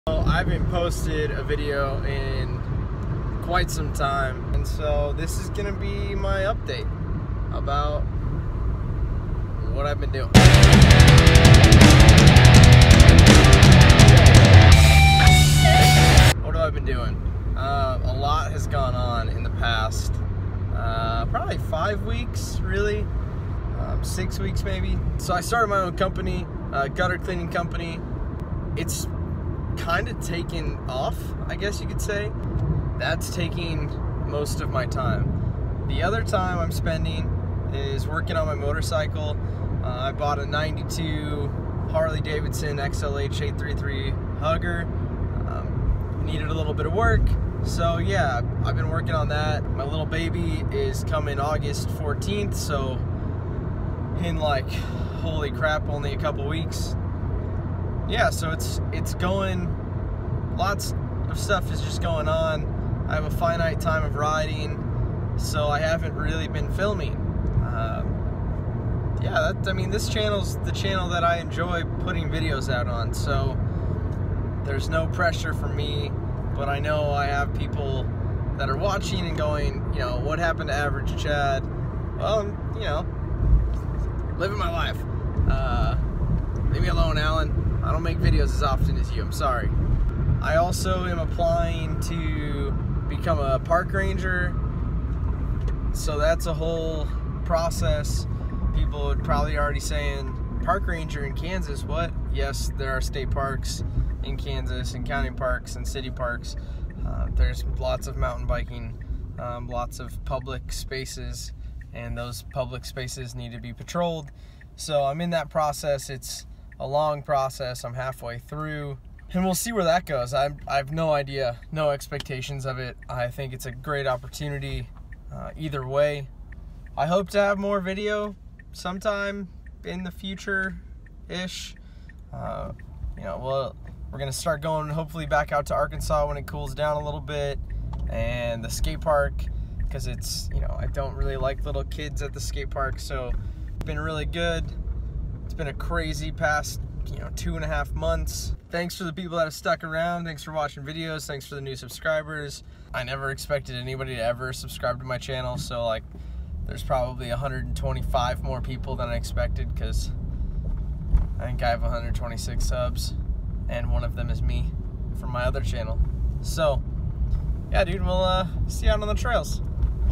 Average Chad UPDATE!
It's been a really long time! Update about where I've been. Maybe I'll be more consistent in the future. Maybe not. Just wanting to make videos of new trails and not trails I ride all the time.